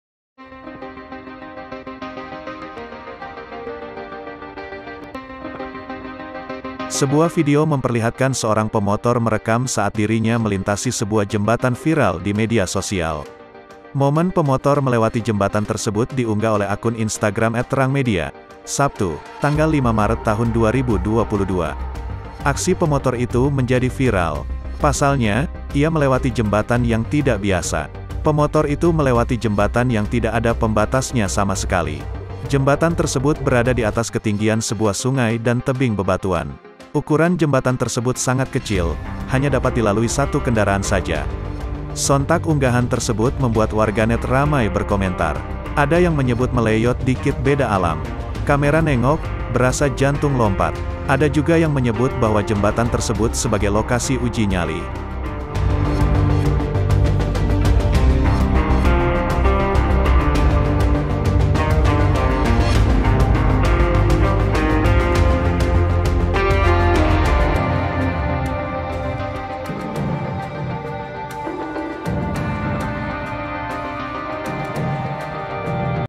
Viral Pengendara Motor Santai Lewati Jembatan Tanpa Pembatas
TRIBUNLAMPUNG - Viral Pengendara Motor Santai Lewati Jembatan Tanpa Pembatas
Momen pemotor melewati jembatan tersebut diunggah oleh akun Instagram @terangmedia, Sabtu (05/03/2022).
Aksi pemotor itu menjadi viral. Pasalnya, ia melewati jembatan yang tidak biasa.
Pemotor itu melewati jembatan yang tidak ada pembatasnya sama sekali.
Jembatan tersebut berada di atas ketinggian sebuah sungai dan tebing bebatuan.
Ukuran jembatan tersebut sangat kecil, hanya dapat dilalui satu kendaraan saja.
Sontak unggahan tersebut membuat warganet ramai berkomentar.
Ada yang menyebut Mleyot dikit beda alam.. Kamera nengok, berasa jantung lompat.
Ada juga yang menyebut bahwa jembatan tersebut sebagai lokasi uji nyali.
Videografer Tribunlampung.co.id / Rio Angga Saputra